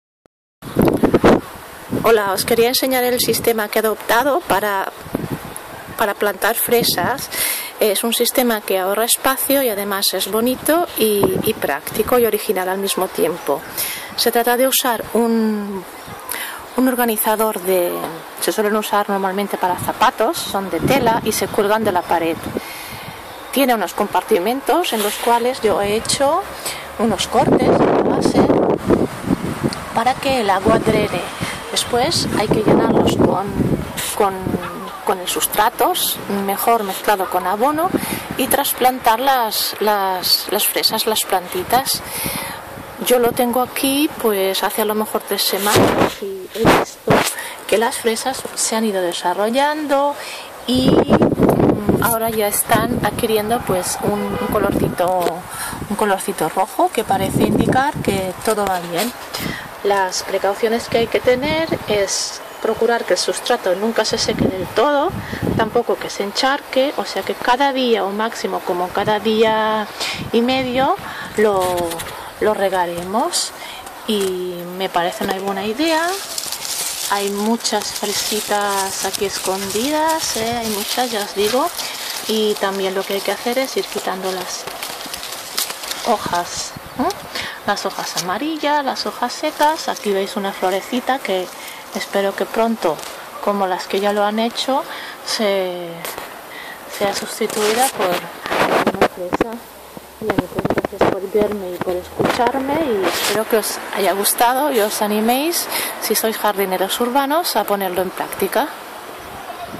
Huerto urbano vertical
Usa un organizador de tela para 20 plantas de fresa. Disculpas por el sonido...